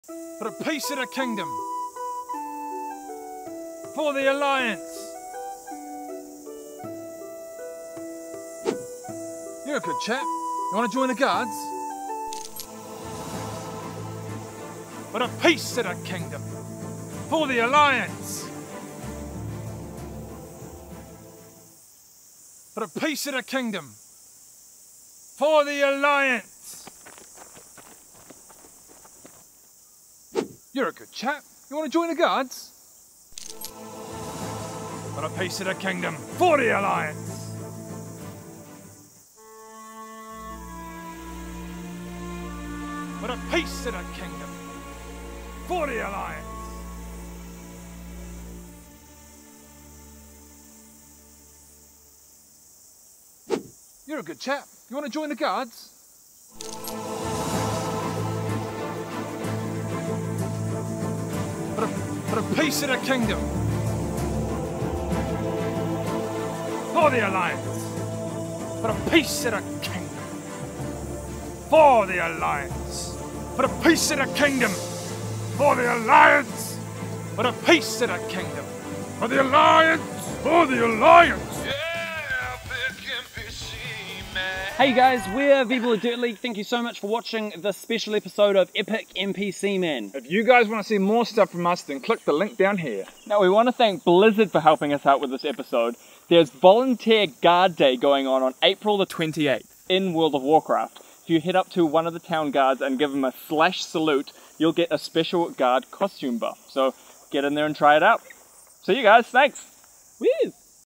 Guard Day is for players only!
It's guard day and Greg the NPC wants to join in the fun